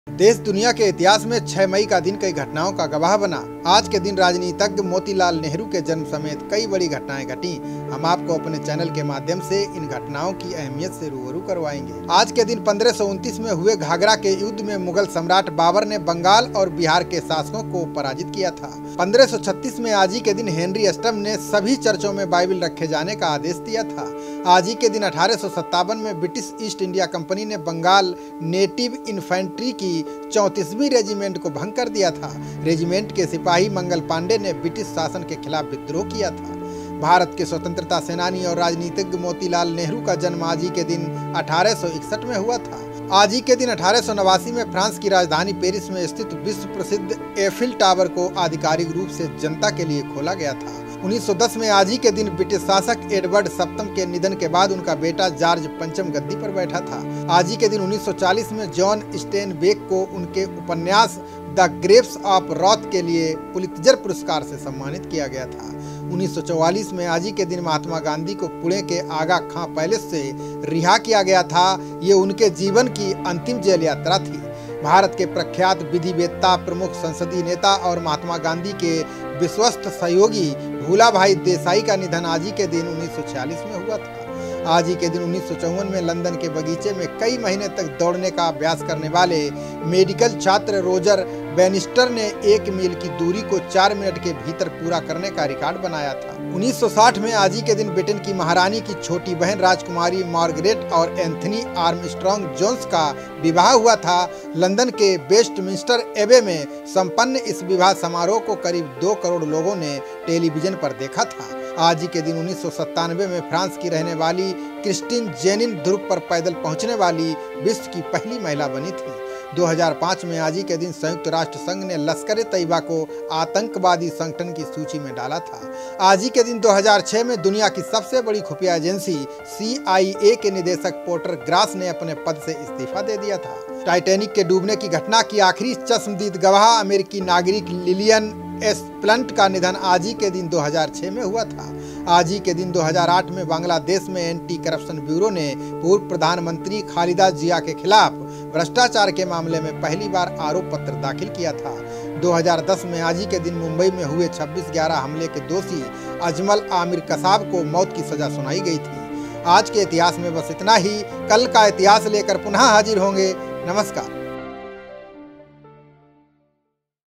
06 May 2020 | आज का इतिहास | Modilal Nehru Birthday | Current Affairs | Today History
देश दुनिया के इतिहास में 6 मई का दिन कई घटनाओं का गवाह बना। आज के दिन राजनीतिक मोतीलाल नेहरू के जन्म समेत कई बड़ी घटनाएं घटी। हम आपको अपने चैनल के माध्यम से इन घटनाओं की अहमियत से रूबरू करवाएंगे।
1-
आज के दिन 1529 में हुए घाघरा के युद्ध में मुगल सम्राट बाबर ने बंगाल और बिहार के शासकों को पराजित किया था।
2-
1536 में आज ही के दिन हेनरी अष्टम ने हर चर्च में बाइबिल रखे जाने का आदेश दिया था।
3-
आज ही के दिन 1857 में ब्रिटिश ईस्ट इंडिया कंपनी ने बंगाल नेटिव इन्फैंटरी की 34वीं रेजिमेंट को भंग कर दिया था। रेजिमेंट के सिपाही मंगल पांडेय ने ब्रिटिश शासन के खिलाफ विद्रोह किया था।
4-
भारत के स्वतंत्रता सेनानी और राजनीतिज्ञ मोतीलाल नेहरू का जन्म आज ही के दिन 1861 में हुआ था।
5-
आज ही के दिन 1889 में फ्रांस की राजधानी पेरिस में स्थित विश्व प्रसिद्ध एफिल टावर को आधिकारिक रूप से जनता के लिए खोला गया था।